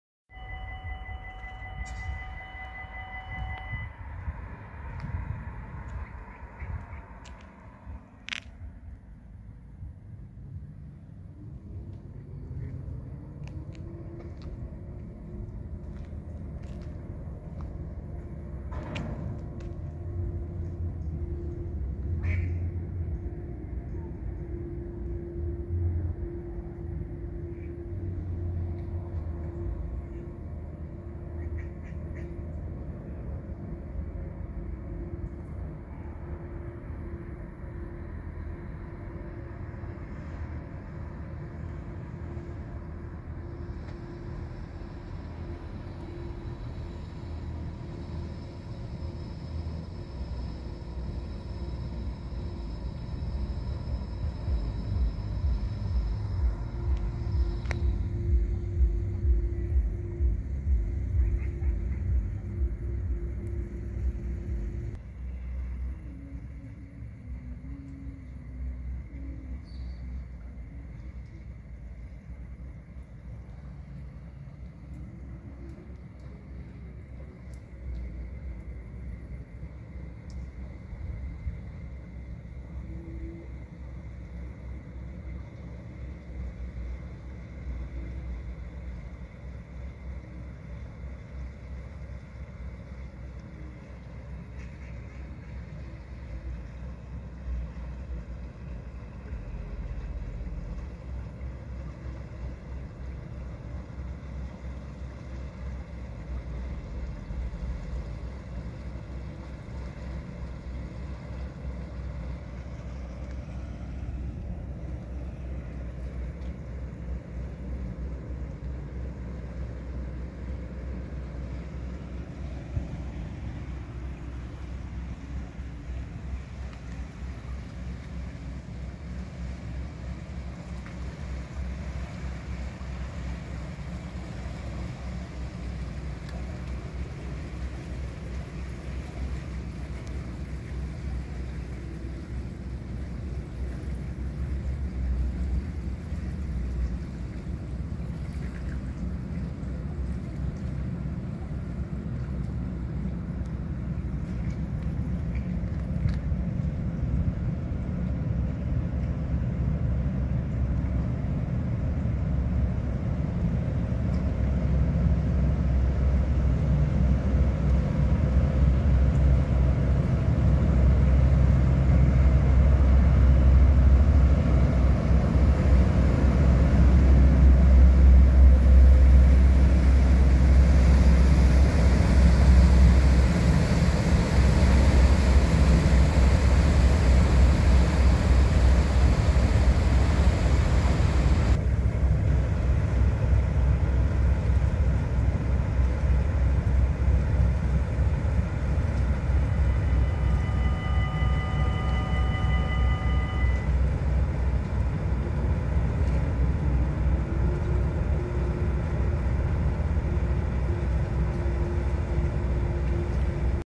Duwboot LEO passeert de WOLDBRUG bij Appingedam.
De Leo passeert de Woldbrug over het Eemskanaal bij Appingedam. Het schip vaart vanuit de richting Groningen naar Delfzijl. (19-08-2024).